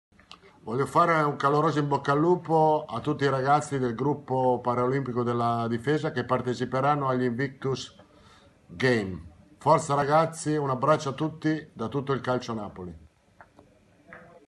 Invictus Games 2018: l'in bocca al lupo di Carlo Ancelotti al GSPD
16/10/2018 - "In bocca al lupo" agli atleti del Gruppo Sportivo Paralimpico della Difesa dai VIP. Approfondisci su